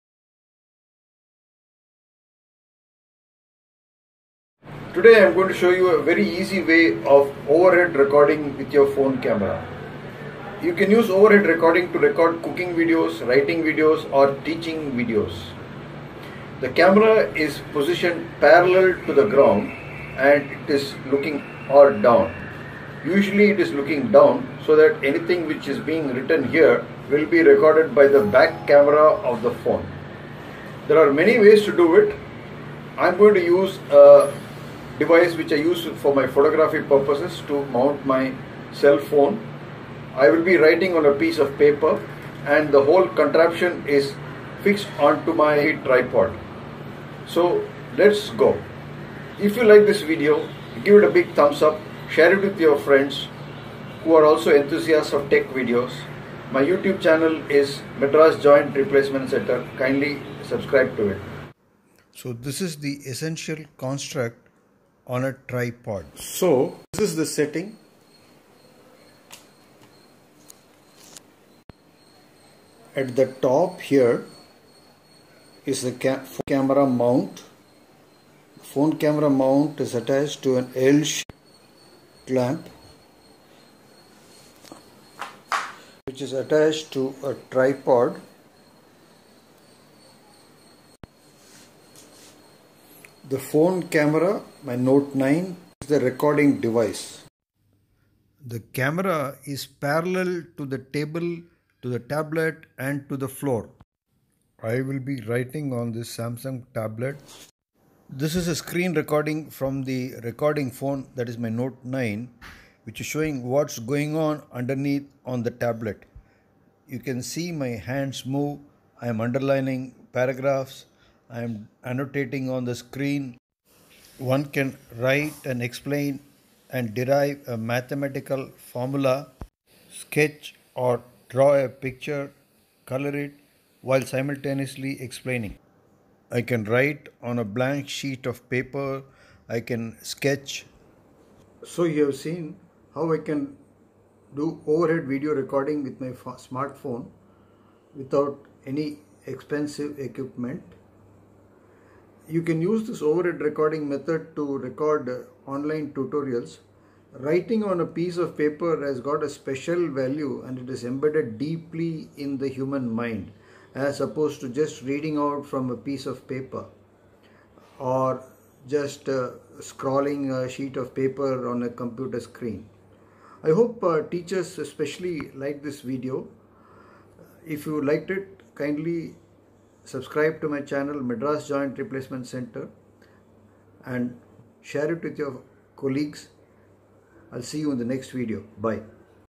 Recording overhead video for online teaching, Dr.A.K.Venkatachalam
An overhead video recording is demonstrated here.  The sight of tracing out the path of a pen on paper to  explain mathematical formula, sketch, colour and draw are deeply embedded in the human brain since childhood. We bring these skills to your home with a readily available smart phone camera.
Your online class will be more absorbing. You can source any material in your house in a creative manner to achieve this like a pile of books, coat hanger,  camera attached to a ladle handle. utensil rack, paper rack, cut out two litre coke bottle.